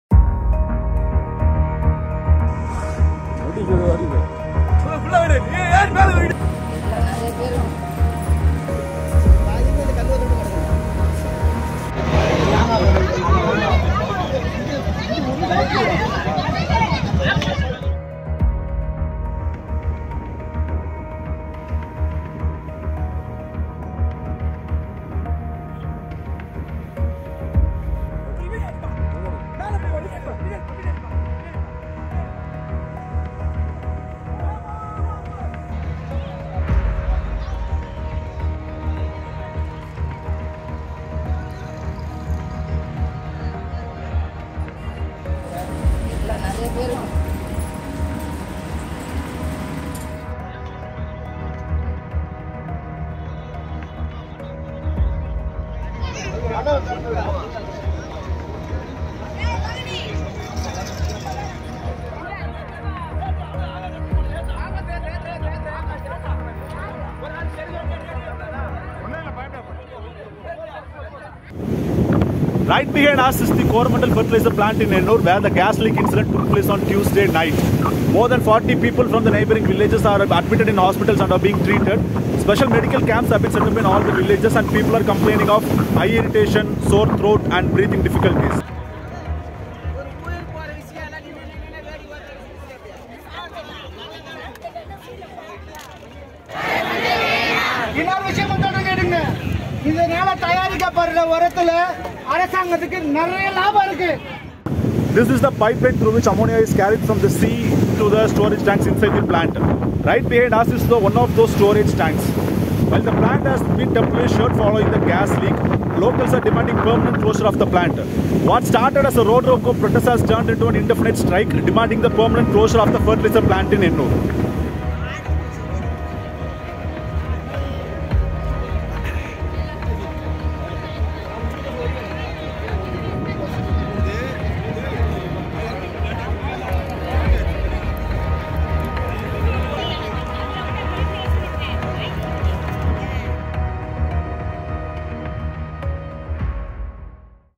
Panic grips Chennai's Ennore as Ammonia gas leaks from a private factory
An incident of Ammonia gas leak from Coromandel International Ltd in Chennai's Ennore, was reported on December 26 night at 12:45 AM. At least 42 people got hospitalized after suffering from breathing difficulties, burning eyes, and throat. A protest by locals is ongoing who are demanding the plant to be permanently shut down.